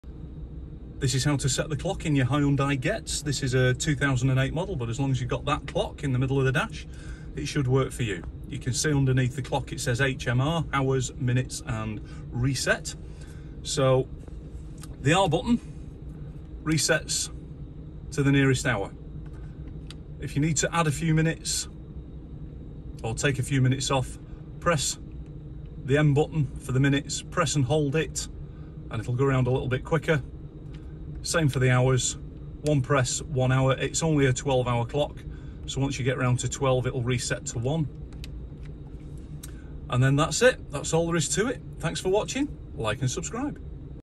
Hyundai Getz Clock Setting how to set the clock in a Hyundai Getz
This is how to set the clock in a Hyundai Getz. This is a 2008 model, but as long as you have got the same clock in the middle of the dashboard, it should work for you. ￼